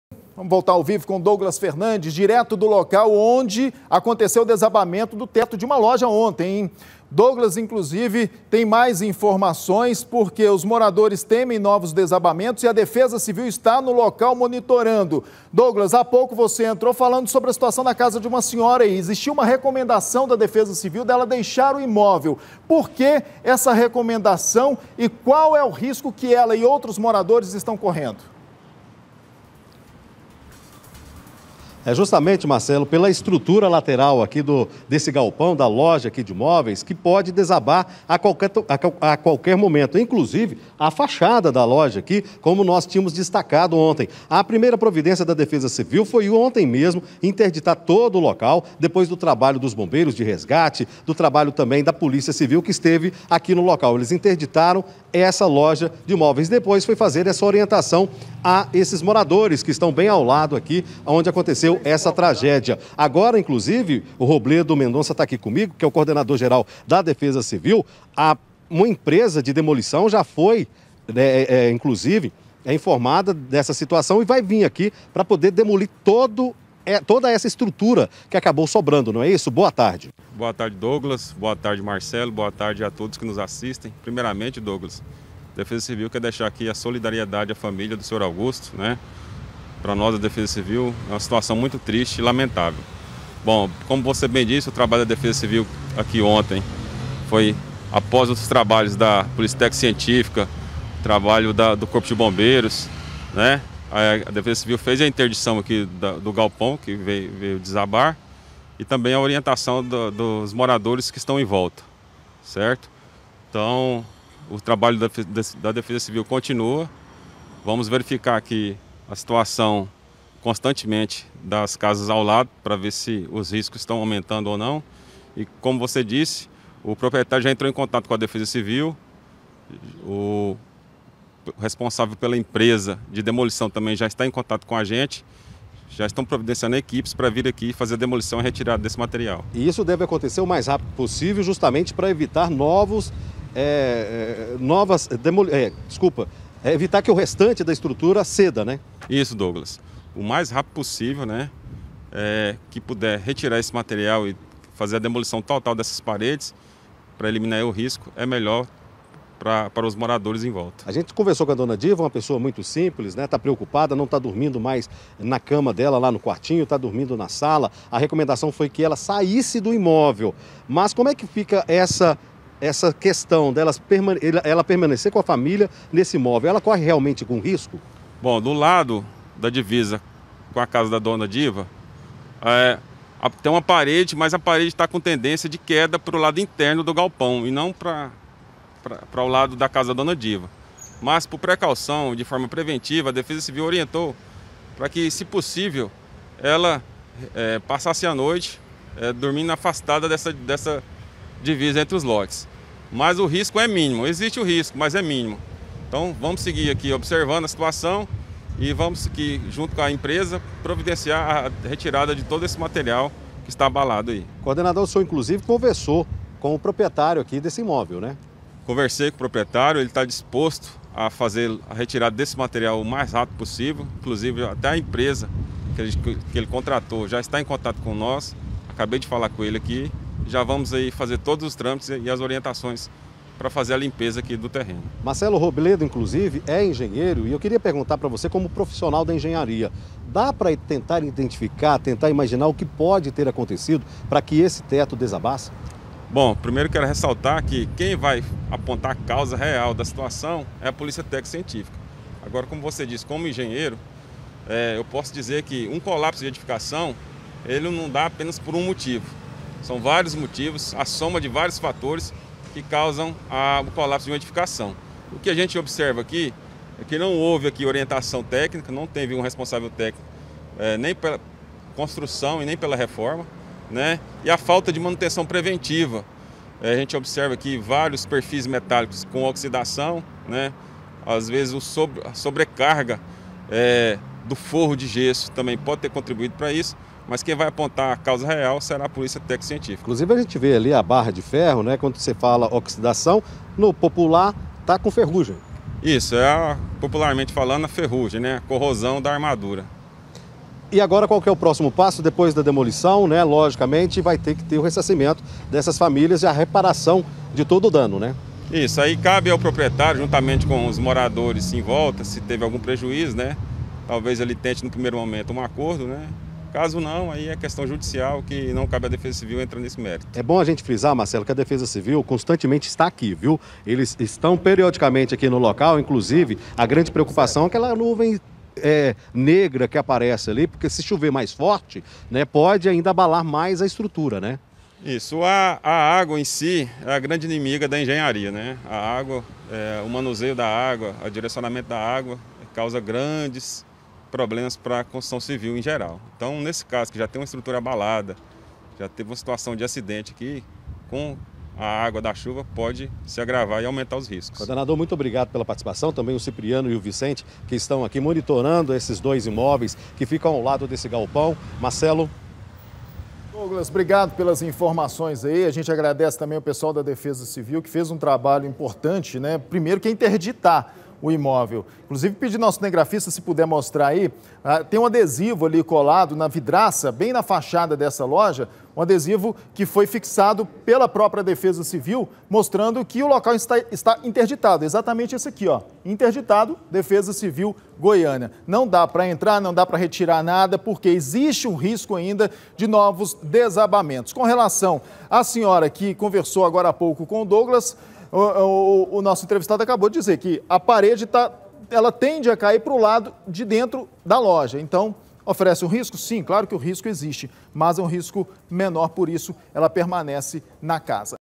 PREOCUPAÇÃO: MORADORES TEMEM NOVOS DESMORONAMENTOS
OS MORADORES DE CASAS EM VOLTA A LOJA DE DECORAÇÃO TEMEM NOVOS DESABAMENTOS.